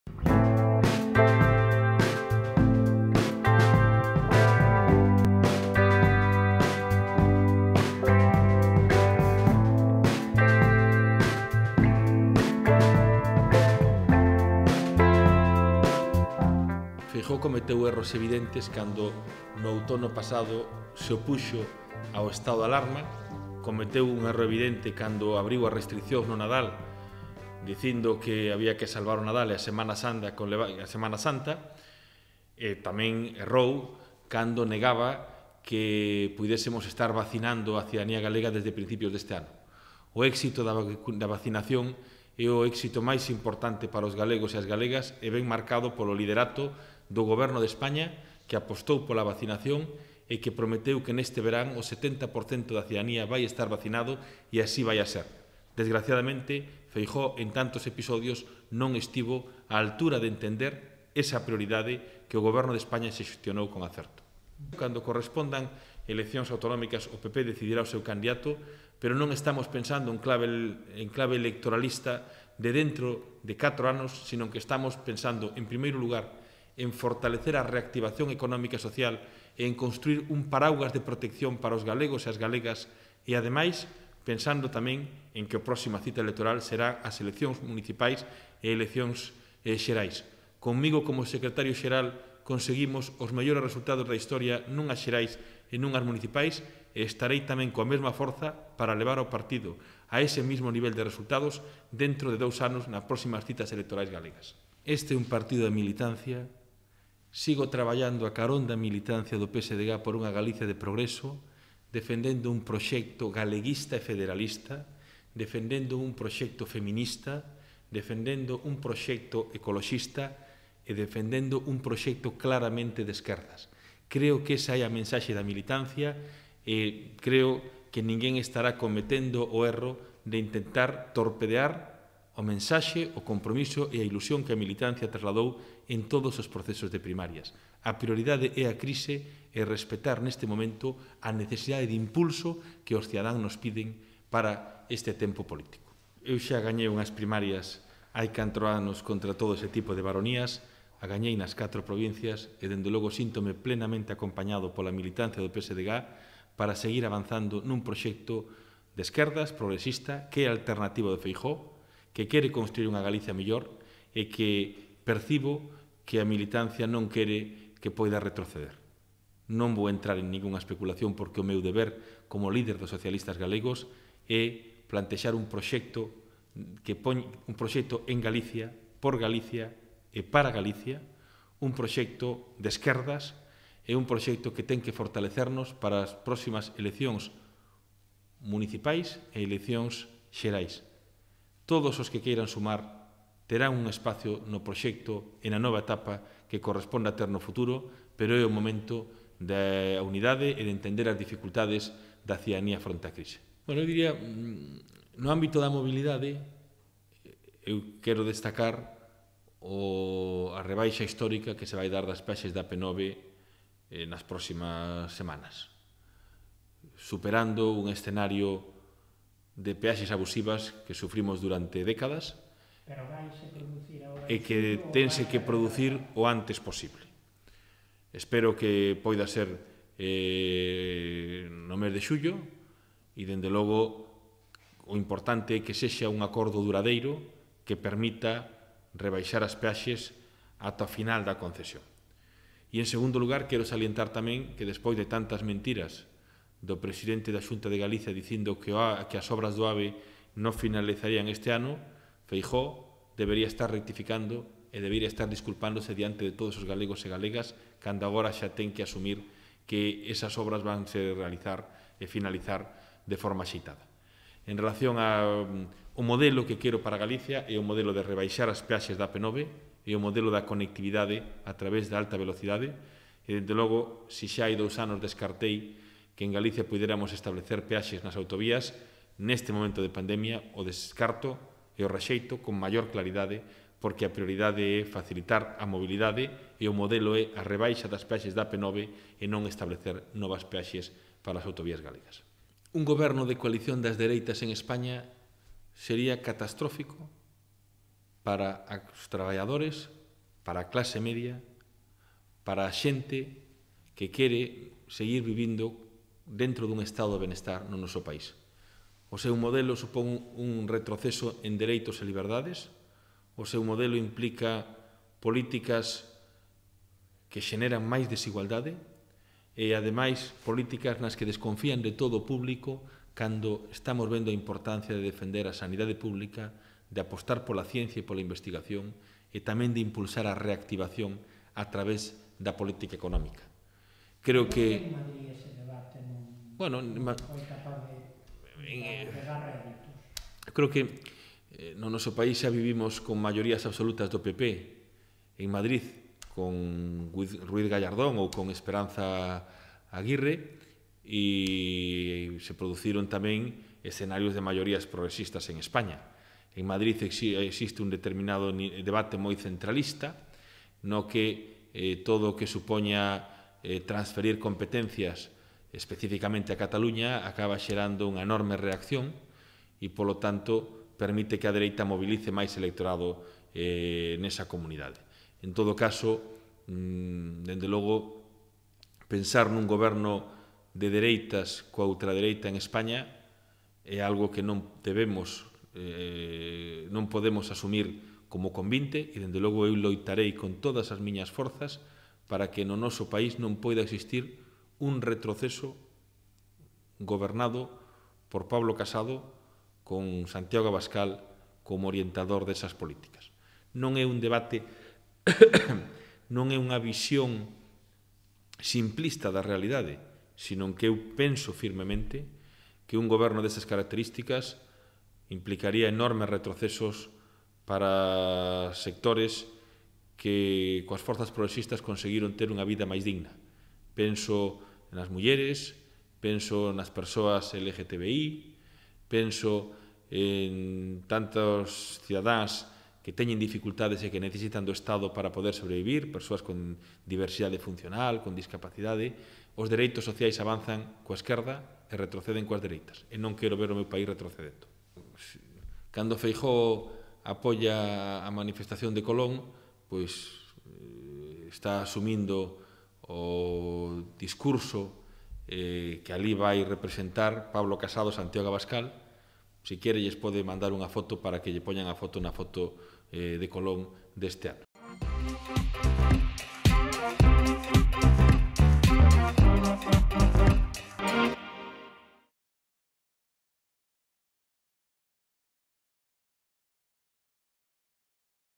Visita de Gonzalo Caballero á sede de El Correo Gallego
O secretario xeral do PSdeG, Gonzalo Caballero, visitou esta semana a sede de El Correo Gallego e aproveitou para contestar preguntas sobre a actualidade do país e do seu partido.